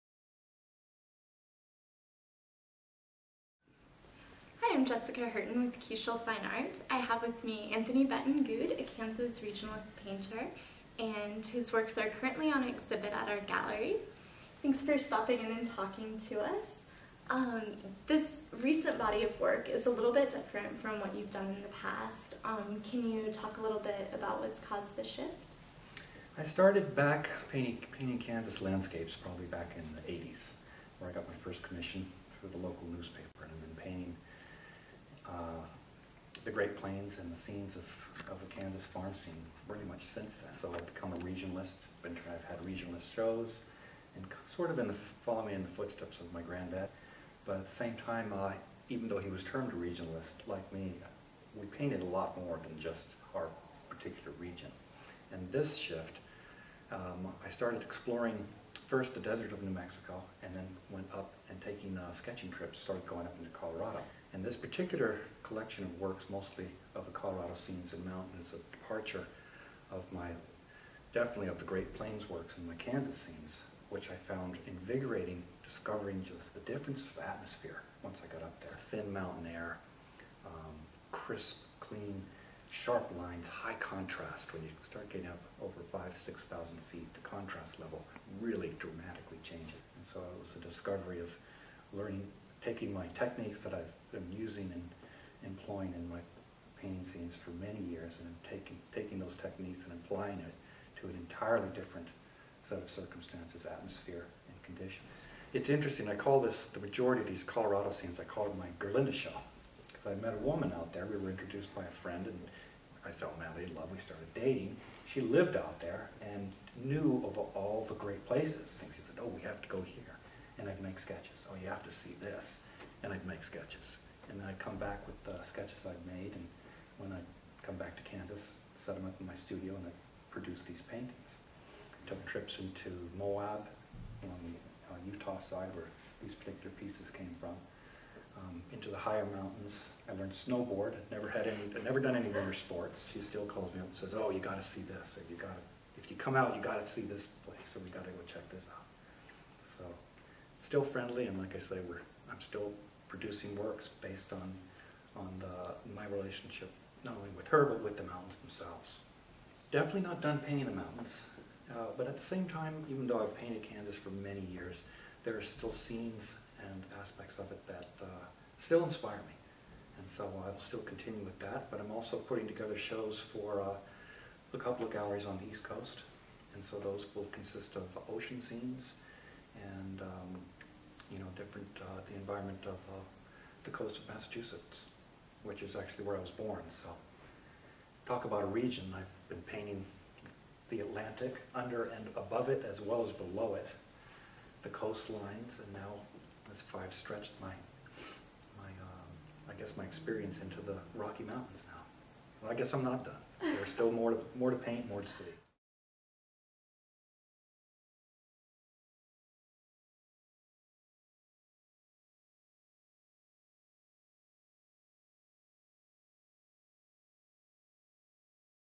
Kiechel Fine Art interviews Anthony Benton Gude
Regionalist Anthony Benton Gude discusses his new body of work.  Known for Kansas scenes, the artist has recently begun exploring the deserts of New Mexico and Utah and the snow-capped mountains of Colorado.

His work will be on display at Kiechel Fine Art through April 8.  His work will be accompanied by drawings and paintings by his grandfather, Thomas Hart Benton, and paintings by fellow Regionalist Dale Nichols.